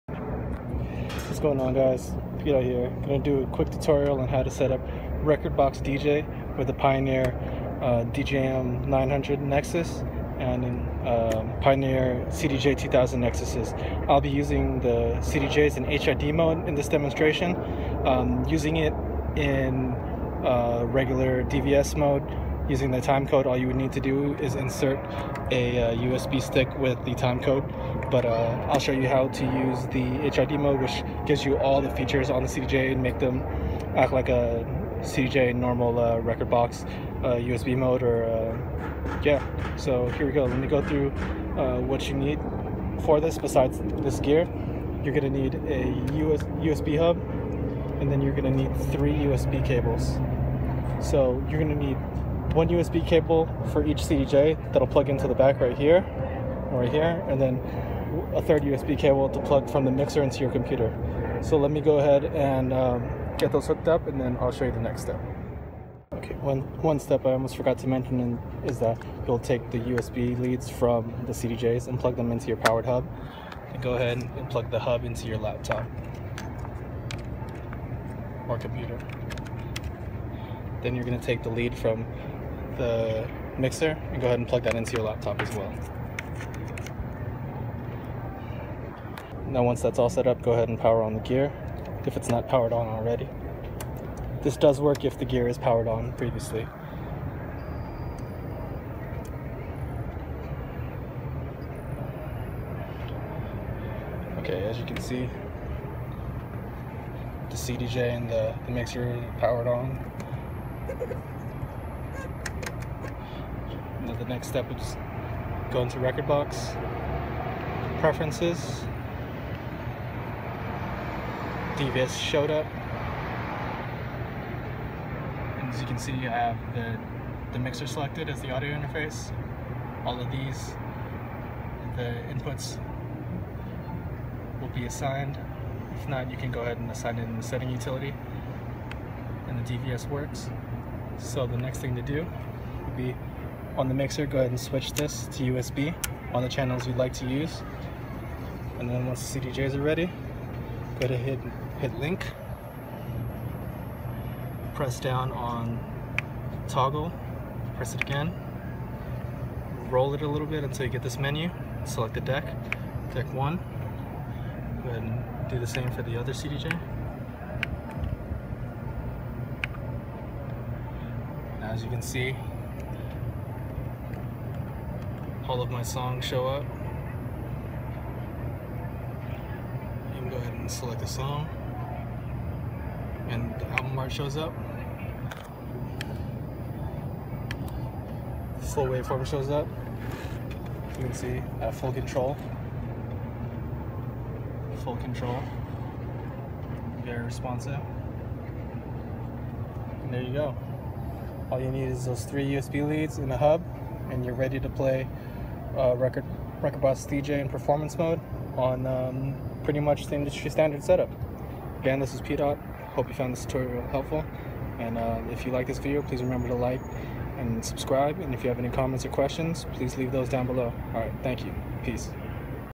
Rekordbox DJ - HOW TO SETUP HID MODE
Quick step by step guide to setting up HID mode for Rekordbox DJ. HID mode makes the Rekordbox DJ experience super responsive, as well as lets you use all the features on the CDJ directly.

New content hits YouTube every Friday!